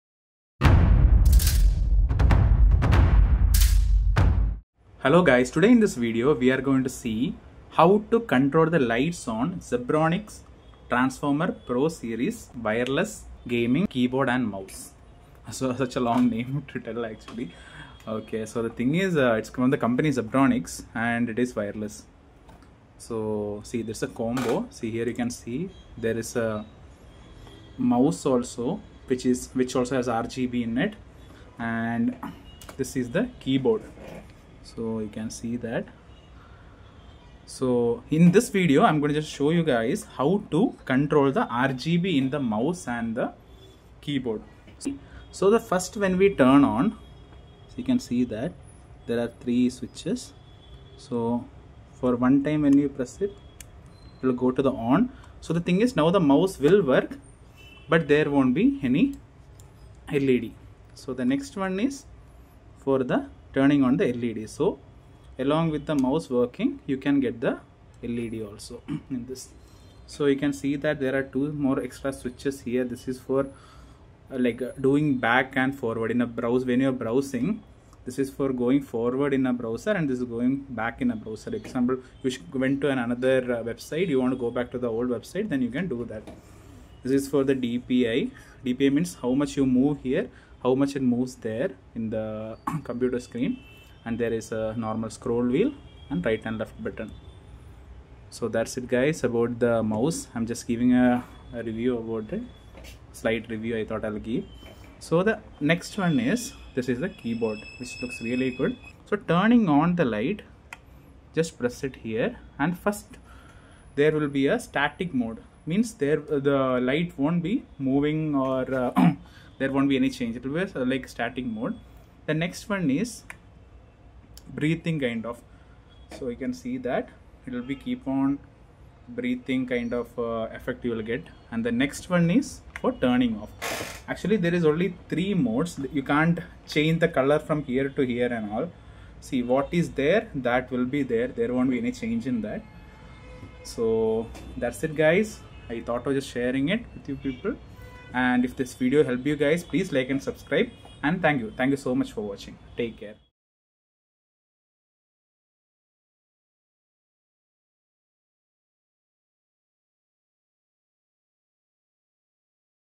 Zebronics Transformer Keyboard RGB Light Settings
In this video I will show you guys how to control RGB light on Zebronics Transformer Pro Wireless Keyboard and Mouse.